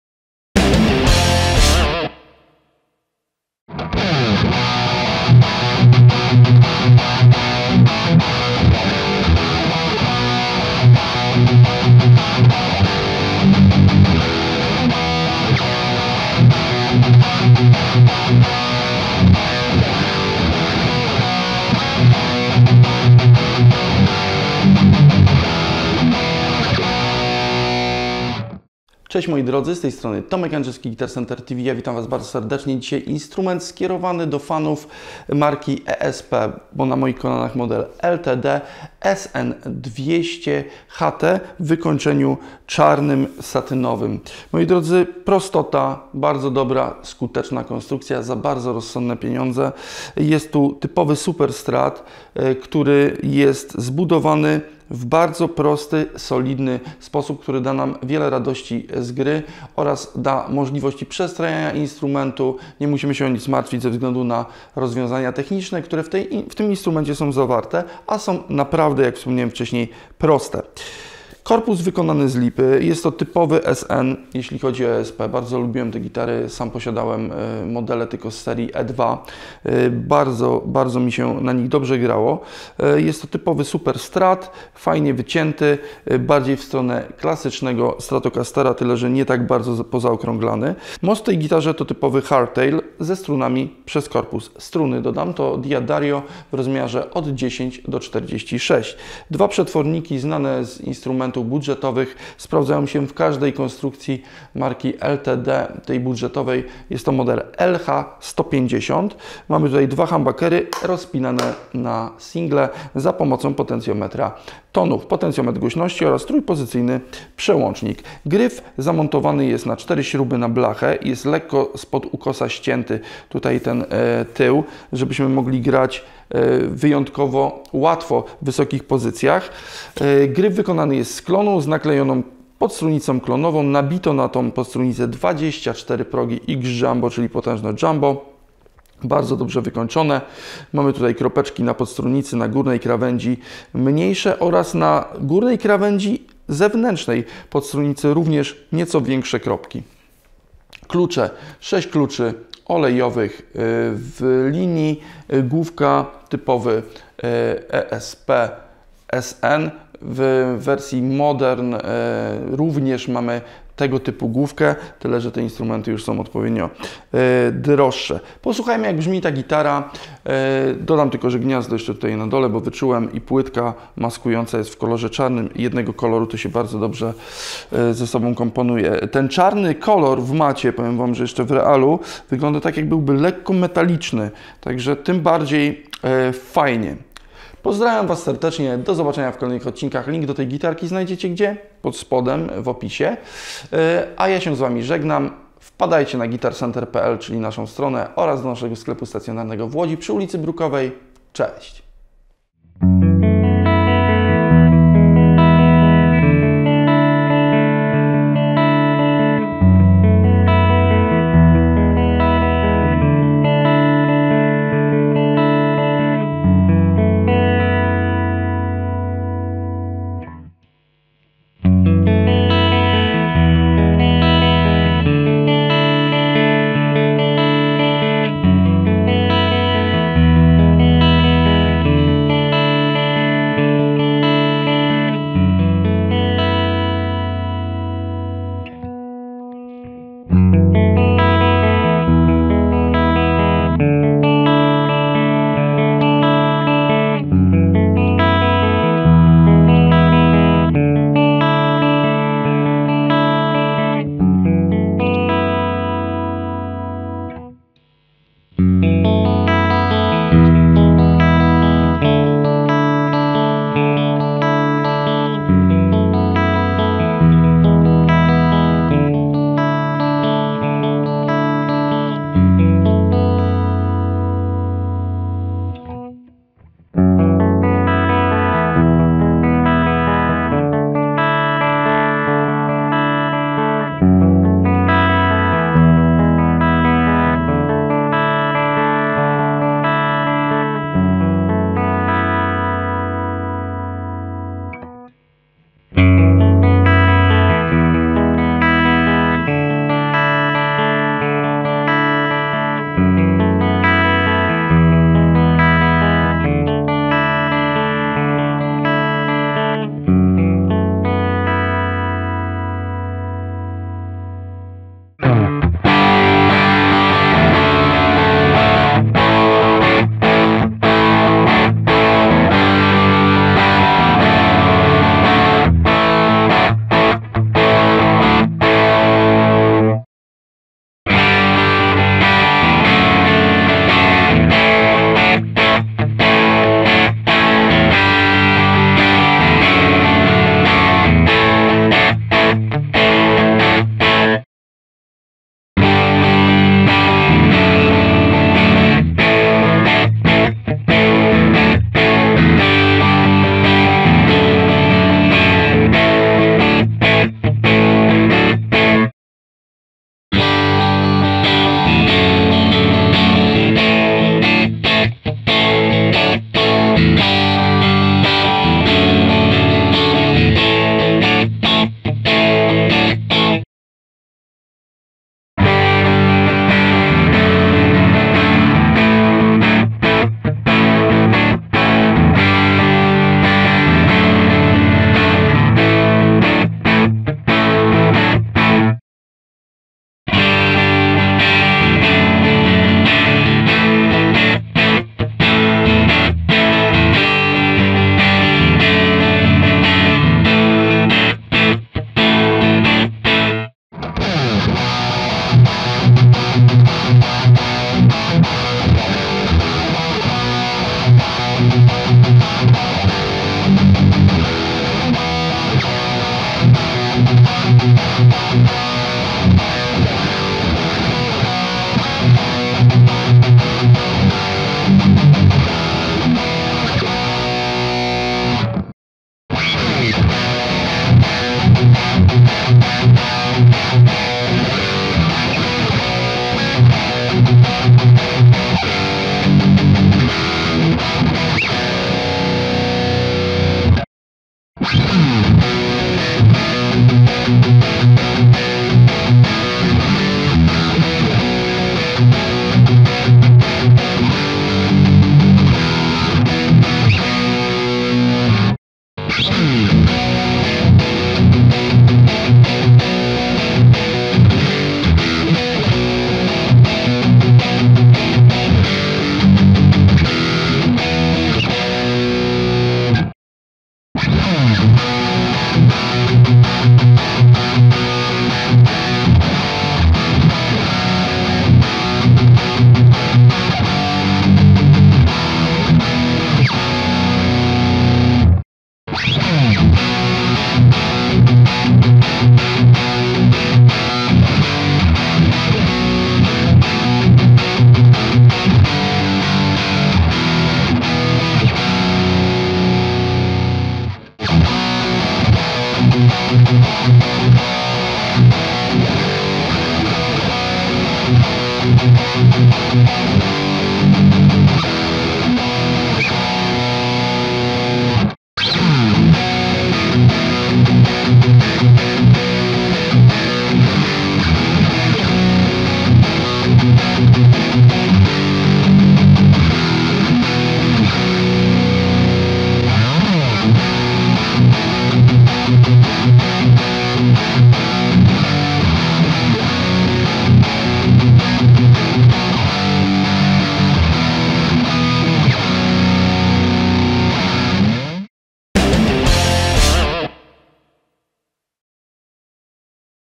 ESP LTD SN-200HT CMS | TV Guitar Center
Do nagrań gitary użyliśmy:
- wzmacniacz: Victory Amplifiers VC35 The Copper (kanał czysty pod zewnętrzne efekty)
- efekty: Line 6 HX Stomp ( jako efekty, przestrzenne, modulacyjne oraz overdrive i distortion)